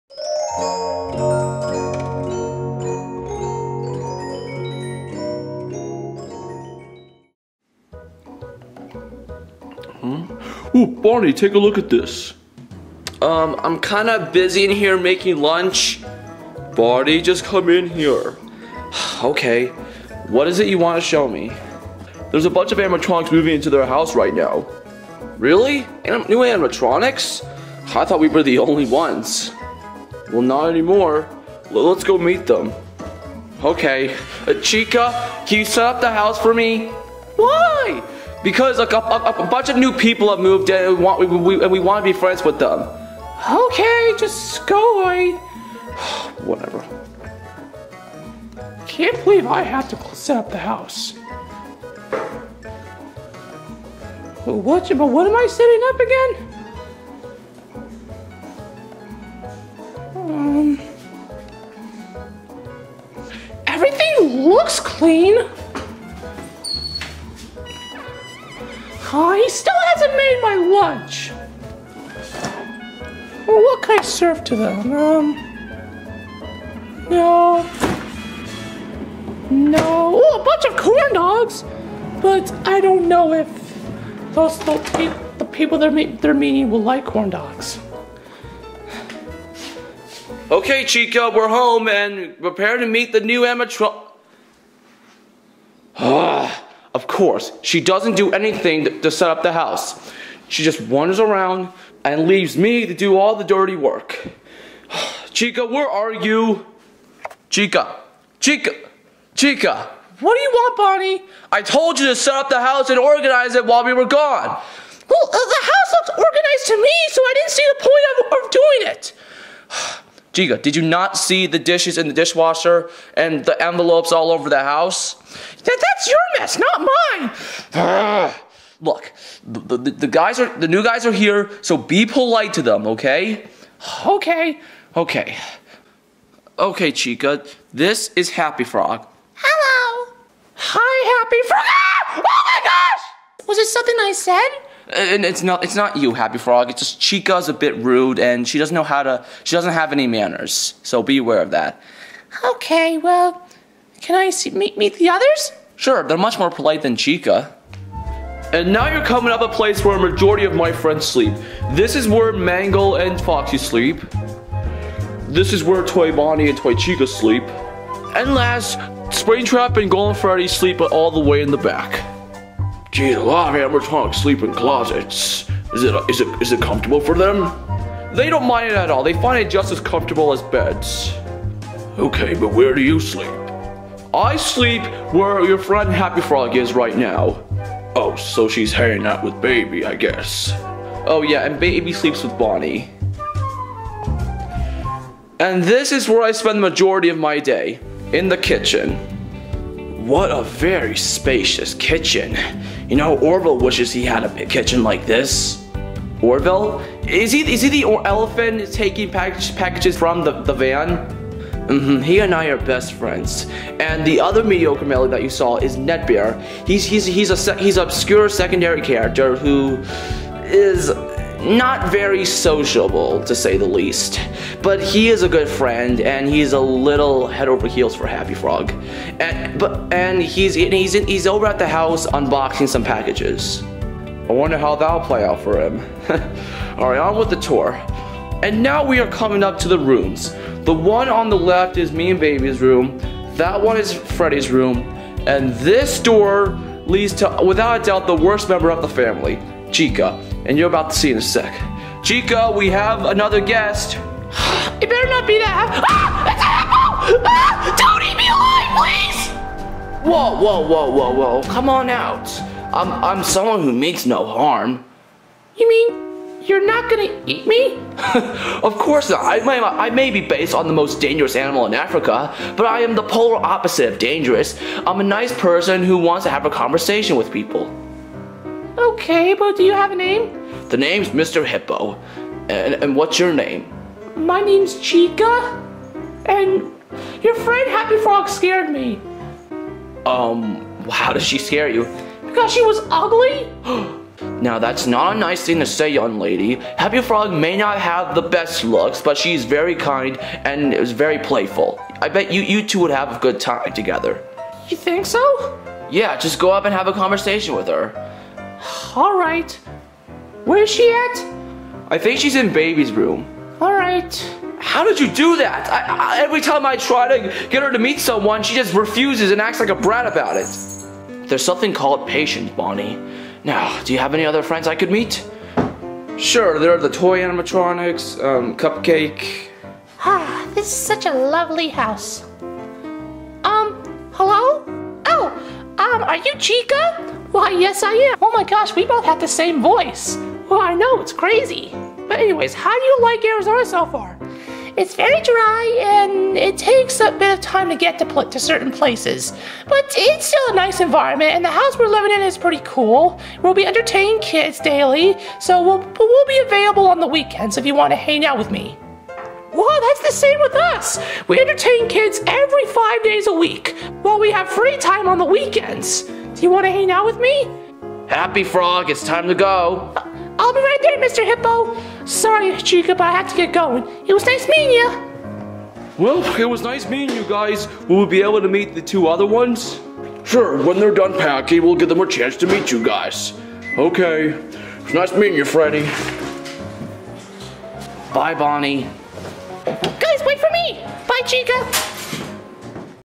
FNaF Short #13 The Mediocre Melodies
A bunch of new animatronics had just moved into the FNaF 1 gang's neighborhood!